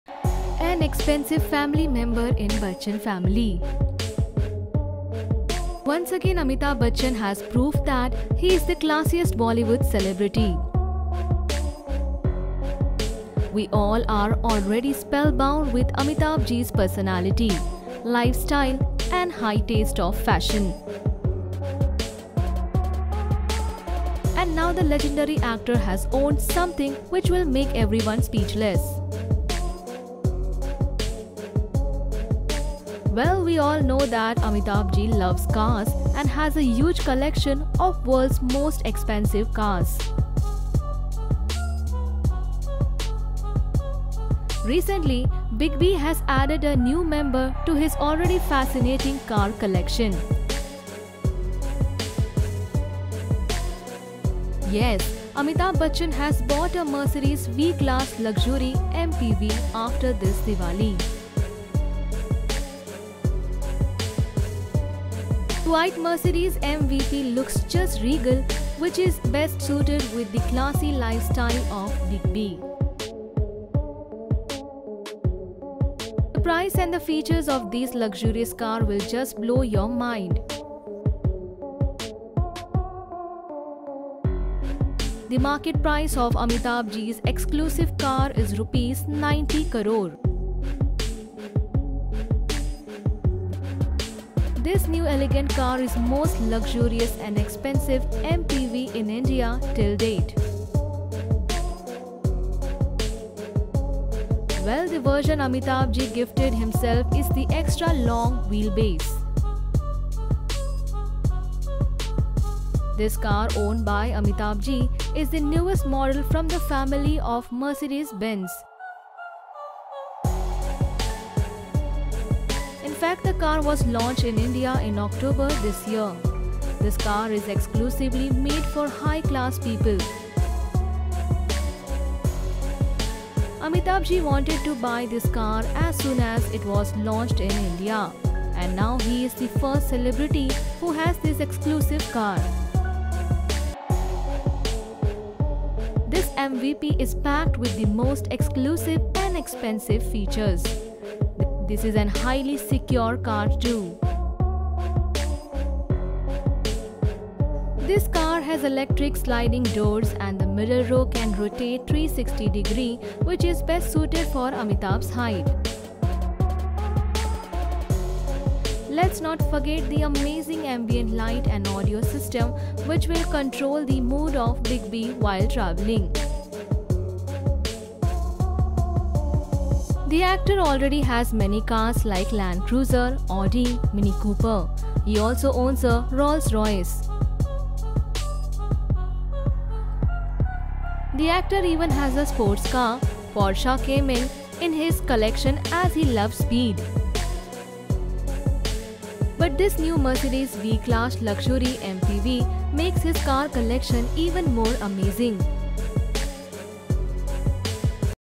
New Family Member in Amitabh Bachchan Family
An Expensive Family Member in Bachchan Family. Once again Amitabh Bachchan has proved that he is the classiest Bollywood celebrity. We all are already spellbound with Amitabh Ji’s personality, lifestyle and high taste of fashion. And now the legendary actor has owned something which will make everyone speechless. Well, we all know that Amitabh Ji loves cars and has a huge collection of world’s most expensive cars. Recently, Big G has added a new member to his already fascinating car collection. Yes, Amitabh Bchchan has bought a Mercedes V-Class luxury MPV after this diwali . The white Mercedes MVP looks just regal which is best suited with the classy lifestyle of BigB. The price and the features of this luxurious car will just blow your mind!. The market price of Amitabh Ji’s exclusive car is Rs. 90 Crore. This new elegant car is the most luxurious and expensive MPV in India till no . Well, the version Amitabh Ji gifted himself is the Extra Long Wheelbase. This car owned by Amitabh Ji is the newest model from the family of Mercedes-Benz. In fact the car was launched in India in October this year. This car is exclusively made for high class people . Amitabh Ji wanted to buy this car as soon as it was launched in India and now he is the first celebrity who has this exclusive car. This MVP is packed with the most exclusive and expesnive features. This is an highly secure car too . This car has electric sliding doors and the middle row can rotate 360 degree which is best suited for Amitabh’s height. Let’s not forget the amazing ambient light and audio system which will control the mood of BigB while travelling. The actor already has many cars like Land Cruiser, Audi, Mini Cooper. He also owns a Rolls Royce. The actor even has a sports car, Porsche Cayman in his collection as he loves speed. But this new Mercedes V-Class luxury MPV makes his car collection even more amazing

Reporters : Srabanti Roy and Bristilekha Roy
Editor : Subrata Gayen  ||  Puja Mitra  ||  Jhilik Das